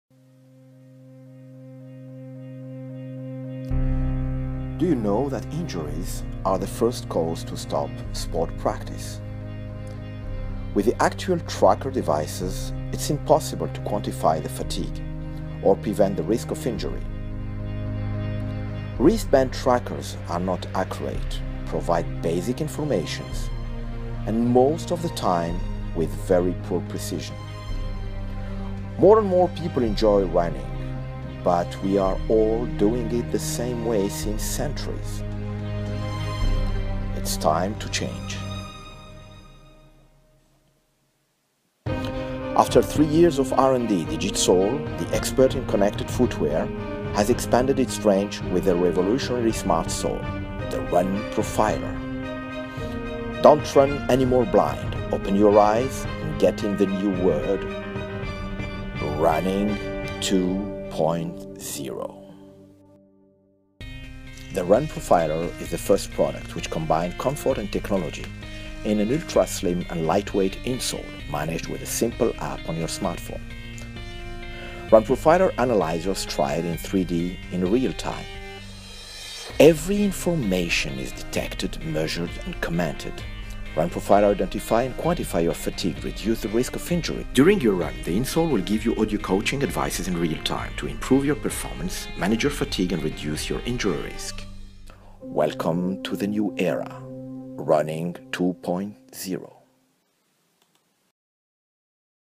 Digitsole Run profiler – din interaktiva, digitala löpcoach
Run Profiler är Digitsoles senaste produktlansering efter den interaktiva värmesulan Warm Series. Innersulan fungerar som en digital löpcoach som ger personlig och direkt feedback så att du kan prestera bättre under löprundan.

Run Profiler analyserar noggrant dina fötters rörelser och din aktivitet och kan direkt upptäcka och kvantifiera din trötthetsgrad, begränsa risken för skador och hjälpa dig förbättra din prestation.

Den nya tekniken är smart förpackad i en lätt, tunn och bekväm innersula som kopplas samman med en app i din smartphone. Dataöverföringen från sulan till sin smartphone sker via Bluetooth Smart, och appen finns tillgänglig för Android och iOS.